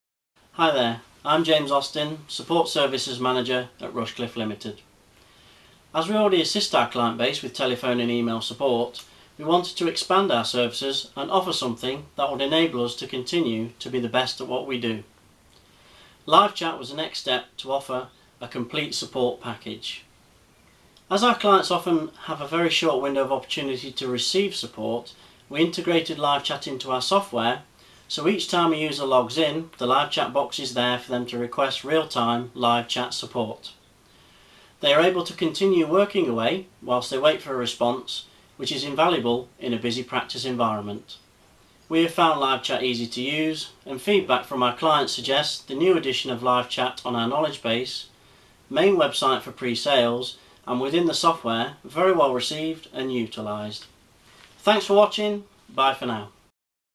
How to get positive customer feedback | LiveChat
James Austin from Rushcliff gives a quick rundown on their use of LiveChat.

Rushcliff placed LiveChat in one of their applications to offer immediate support for their clients.